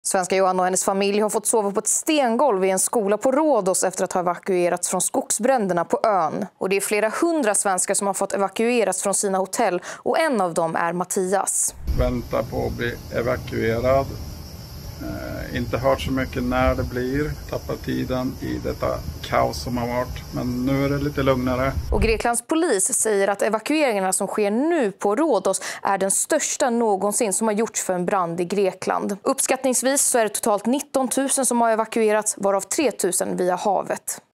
Svenskar evakueras från Rhodos efter kraftiga skogsbränder – familjen sov på ett stengolv
I flera dagar har bränderna härjat på den grekiska turistön Rhodos och tusentals personer har evakuerats. Den största evakueringen i Greklands historia pågår just nu på Rhodos.
Över 19 000 människor har hittills evakuerats och brandmännen kämpar på tre fronter mot lågorna.
I södra delarna av ön har nödläge utfärdats.